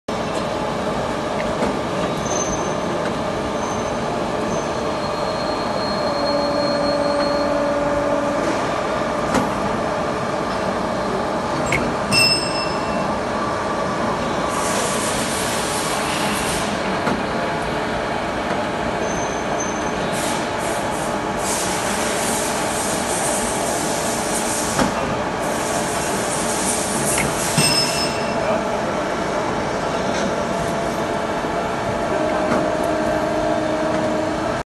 WEEREN Endenbearbeitungsmaschine/End finishing machine DH20
WEEREN EBM Typ  DH20 II P
Anwendungsfall: Bearbeitung von Sechskantschrauben
Maschinendaten:
Spanndurchmesser: Ø 10-40 / Rohr 8-80 mm
Werkstücklängen: 150-2000 mm
Spindelhub: max. 80 mm
Stirnsenkleistung/Vollmaterial: max. Ø 40 mm
Motorleistung: max. 7,5 kW
Maschinenanschlusswert: ca. 22 kW
Spannkraft: 2 x 22000 N
Betriebsdruck Hydraulik: 120 bar
Betriebsdruck Pneumatik: max. 6 bar
Baumaße (Länge/Breite/Höhe): ca. 4000/2500/1600 mm
Gesamtgewicht: 3.000 kg